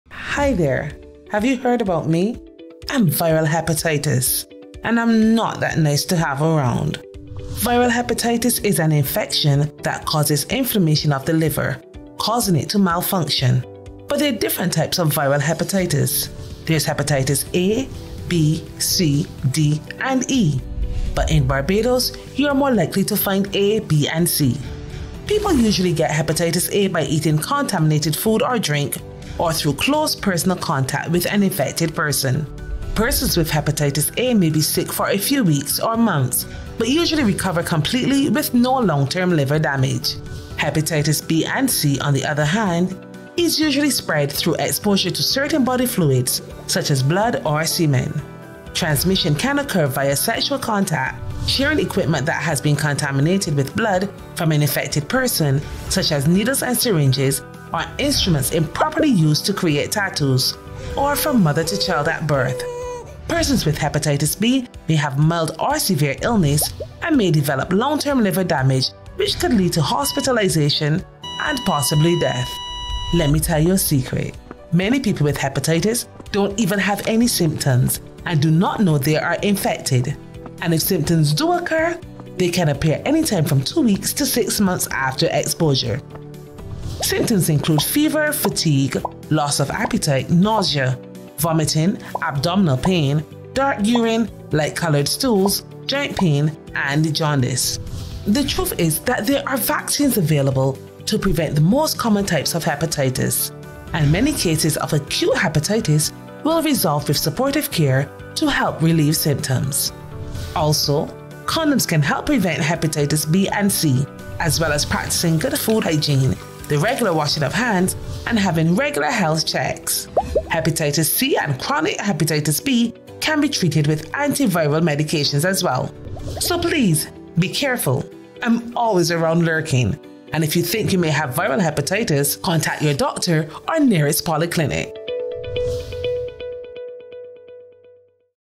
Viral Hepatitis - Be Aware! Get Tested
What is Viral Hepatitis? How can you prevent yourself from contracting it (getting it)?

 Viral hepatitis, particularly hepatitis B virus (HBV) and hepatitis C virus (HCV), is a major cause of disease and mortality worldwide. In the Caribbean, an estimated 240,000 persons are living with HCV.

As we look to a Hepatitis-free future, vaccination, testing and linking to care and treatment is important and cost-effective.